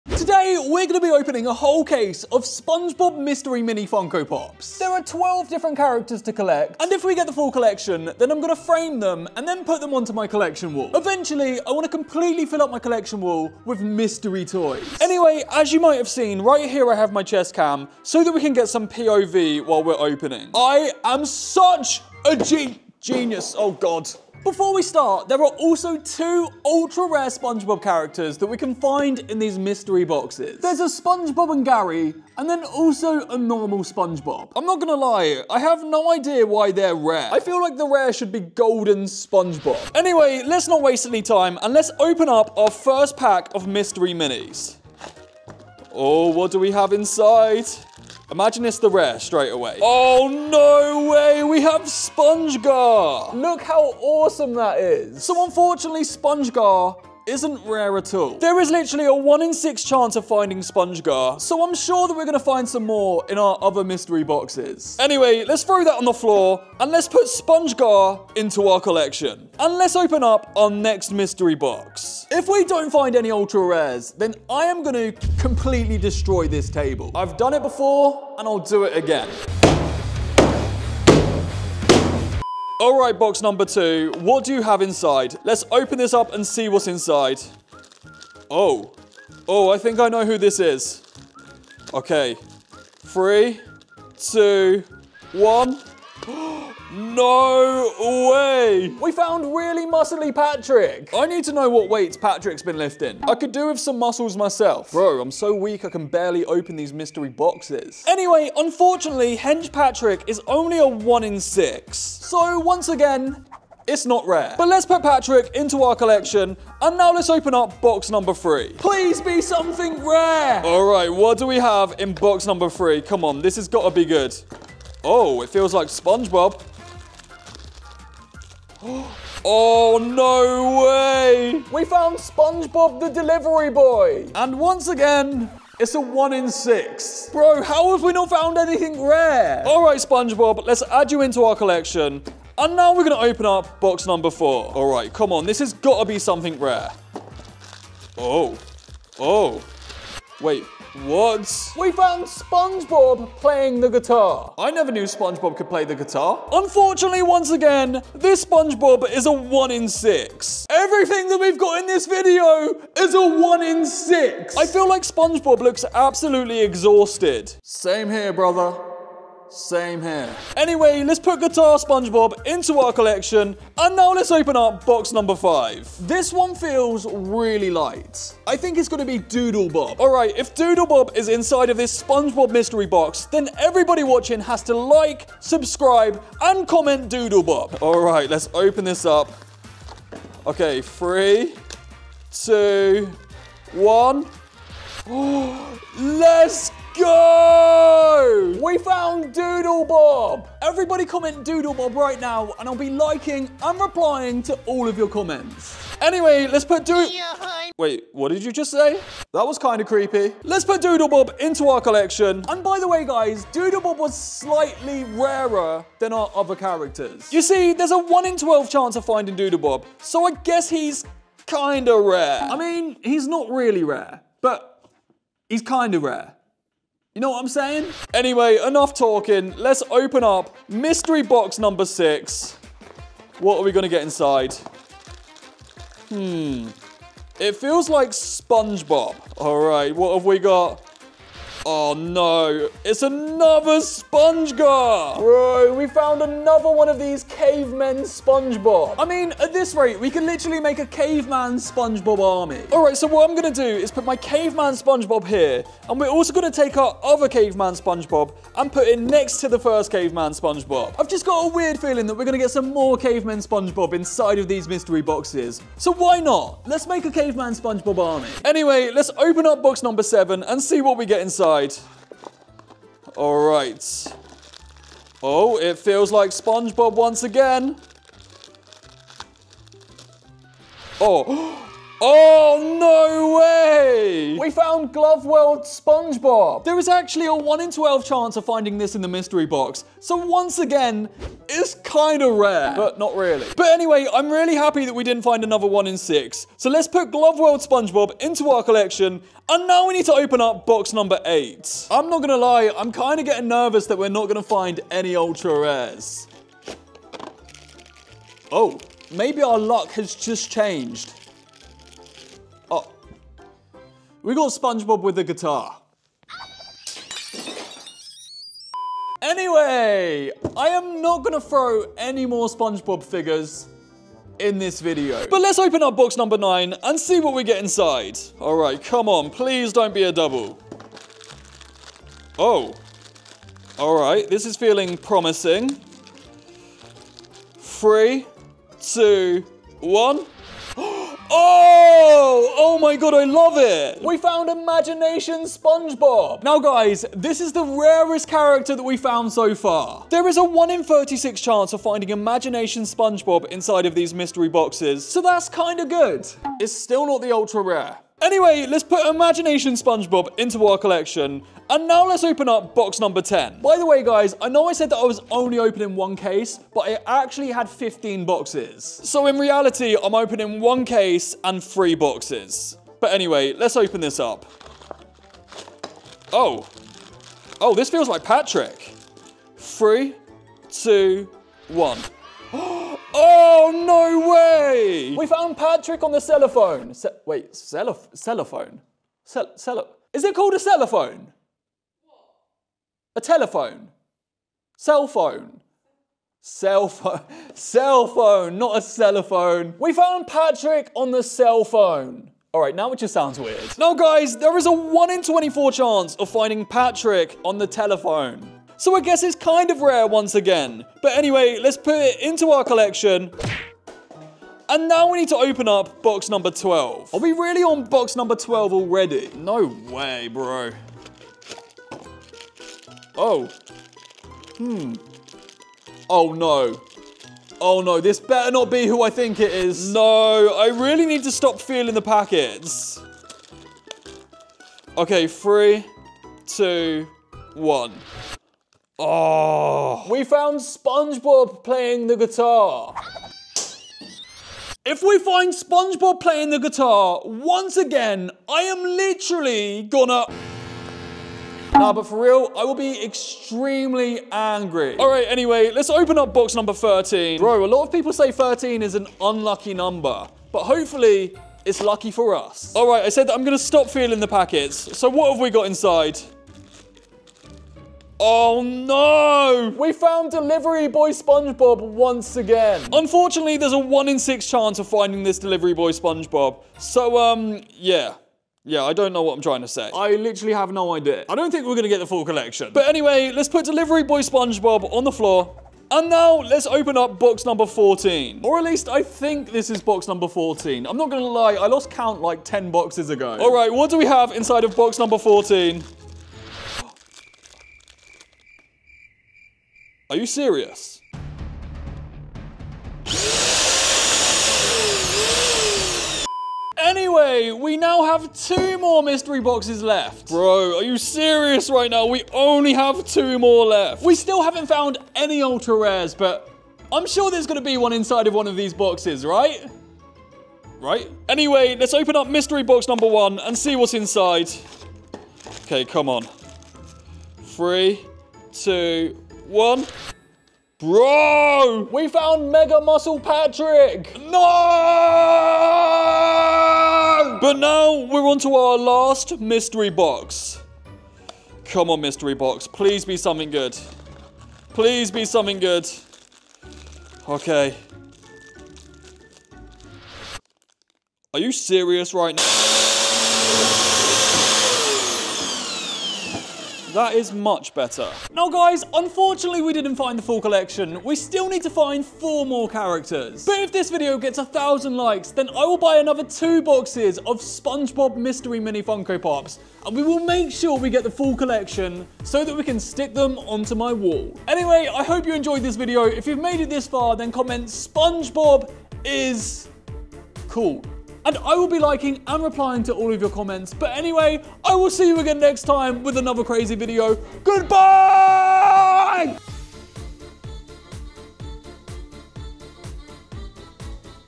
Opening 100 Spongebob Mystery Mini Funkopops (ULTRA RARE)
Today we will be opening 100 Spongebob Mystery Mini Funkopops to try and find the 2 ULTRA RARE SPONEGBOB CHARACTERS and also to try and Complete the whole collection! there are 12 different characters to collect including 2 ULTRA RARES! There is a 1 in 72 chance of finding the ULTRA RARES and today. we only have 15 boxes! I know that the title says opening 100 but I just put that because it sounds much better!